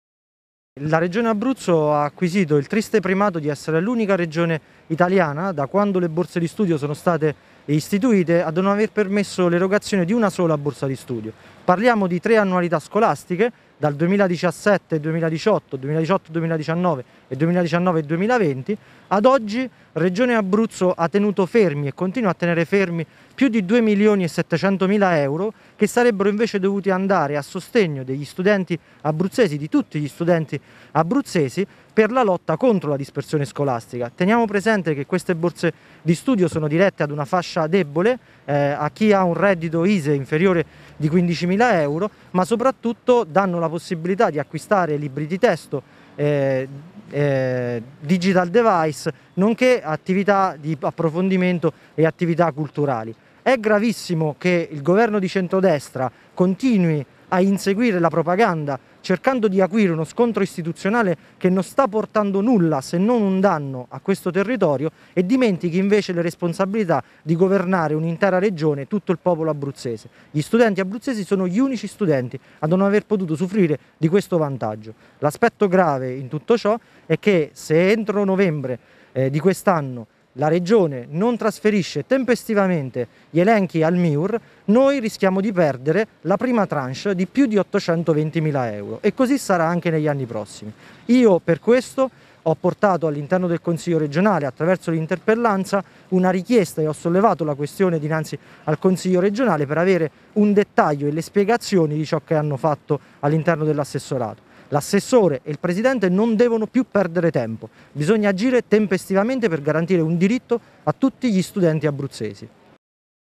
M5S: "Borse d'istruzione non erogate"
L’Abruzzo è l’unica regione in Italia che non ha erogato neanche una borsa di istruzione per gli studenti. Questa la denuncia del M5S che chiede un cambio di passo alla Giunta